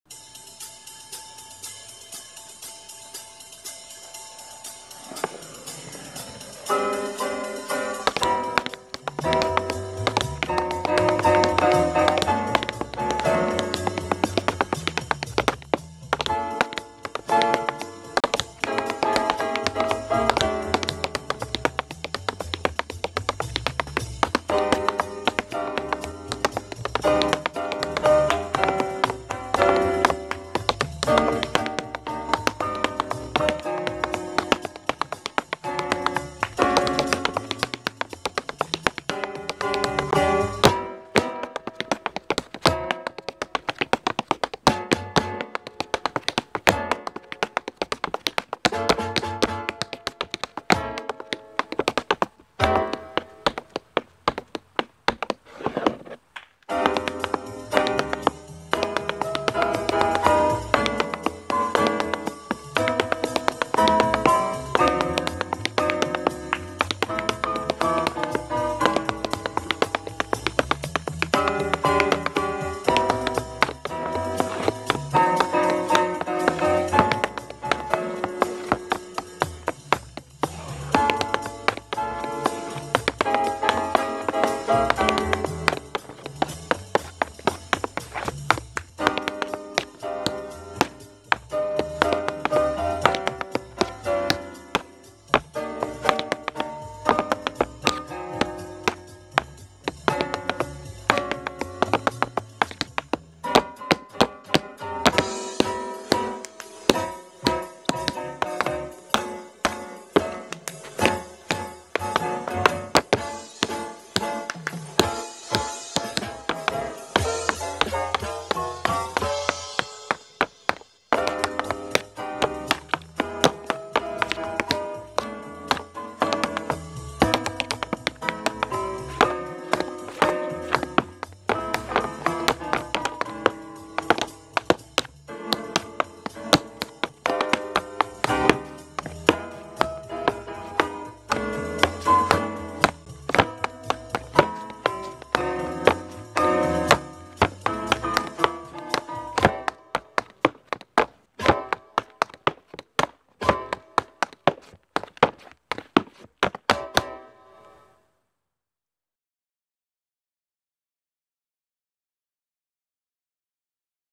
Buster Brown Laura danced by Auksuole
Choreography: Buster Brown
Dancer: Auksuole
Camera & edit: Sofija Lev
Producer: JustTap Dance Company